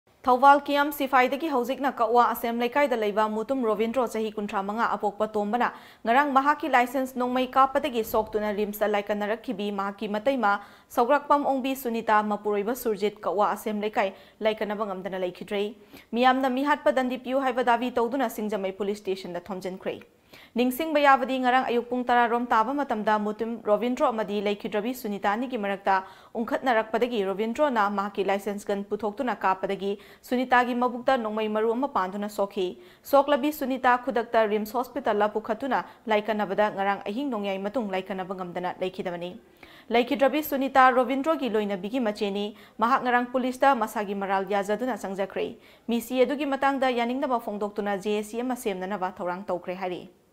Manao nipana nongmeina kapadagi soktuna laikanarakhibi sunita laikannaba ngamdana leikhidre.
Thoubal kiyam shiphaidagi houjikna Kakwa Asem leikai da leiba Mutum Robindro chahi 35, apokpa Tomba na  ngarang mahak ki licensed nongmei  kappadagi soktuna rims ta laikanarakhibi mahaki mateima  Sougrakpam  ongbi  Sunita  ,mapuroiba Surjit , Kakwa Asem Leikai laikanaba ngamdana leikhidre.meeyamna meehatpa dandi piyu haiba dhabi touduna singjamei police station da thomjinkhre.Ningshingba yabadi,ngarang ayuk pung 10 rom taba matamda mutum Robindro amadi lekhidrabi sunita anigi marakta ungkhatnarakpadagi Robindrona makaki Licensed gun puthoktuna kappadagi sunitagi mabukta nongmei maru ama panduna shokhi.soklabi sunita khudakta RIMS hospitalda pukhat tuna laikanbada ngarang ahing nongyai matung laikannaba ngamdana leikhidabani.leikhidrabi sunita Robindro gi loinnabigi macheni.mahak ngarang police ta mashagi maral yajaduna changjakhre.meeshi adugi matangda yaningdaba phongdoktuna JAC ama semnaba thourang toukhre hairi